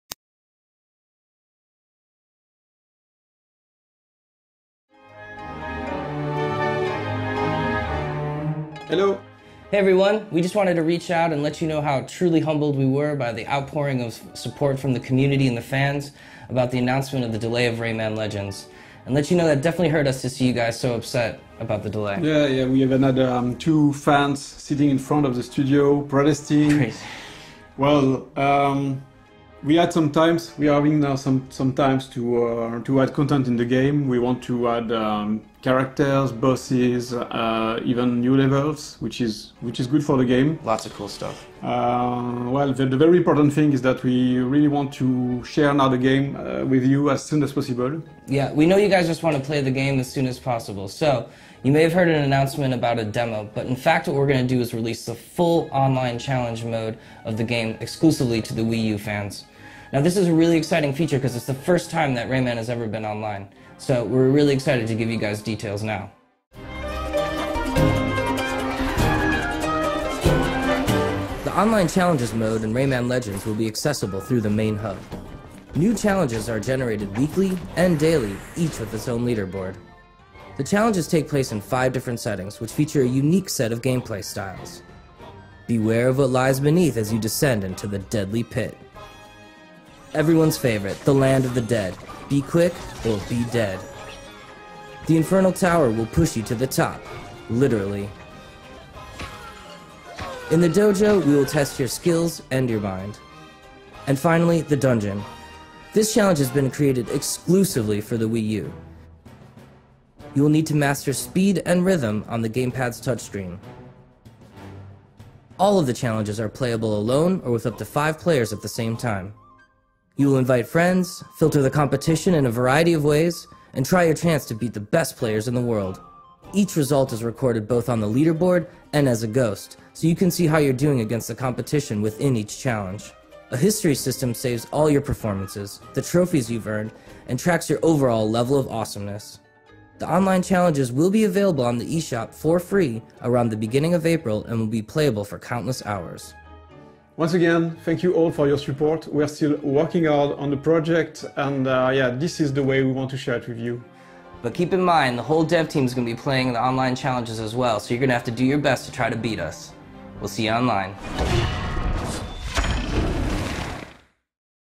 Rayman Legends development update
In response to fan pressure, Ubisoft has announced a challenge mode for Rayman Legends will be available for download in April exclusively on Wii U. It will be made available for all platforms when the game is released in September.